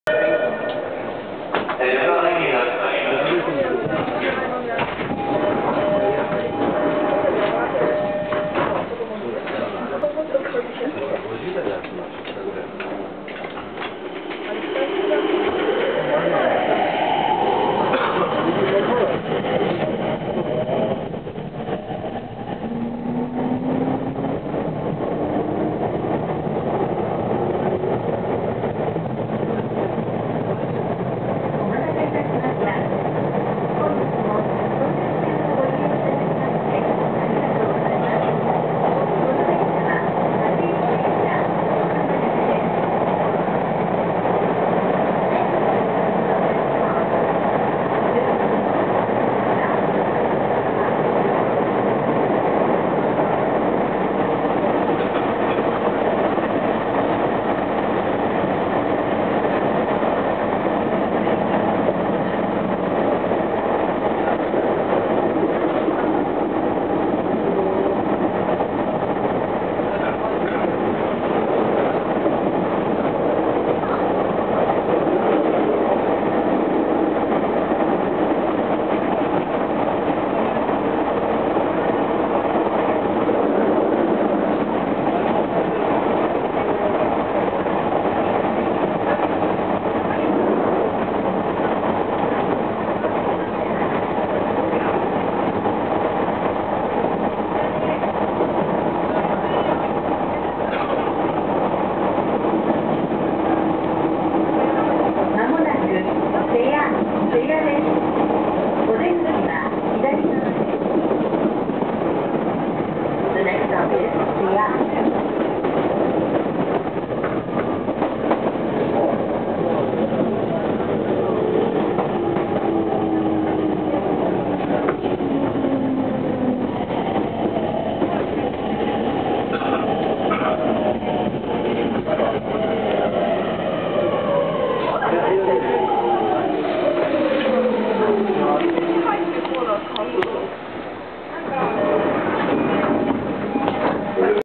相鉄10000系 モハ10105 大和→瀬谷
相鉄10000系の走行音です。

2001年、旧型車両の置換えを目的に登場した車両で、JR東日本E231系の基本的な仕様をほぼそっくりそのまま導入しました。車体幅が2930mm、1M車を導入したことがE231系との違いです。

現在、10両編成3本と8両編成5本の70両が在籍し、8両編成は主に各停中心、10両編成は主に急行・快速などの優等が中心で、運用間に合いで各停に入ることもあります。自動放送は2006年度最終増備車の10708Fを歯切りに全編成、純電気ブレーキは10705F以降の増備車設置を歯切りに全編成装備されました。

この形式からIGBT素子を用いたVVVFインバータ制御装置に変更され、こちらもE231系と全く同じ音を出します。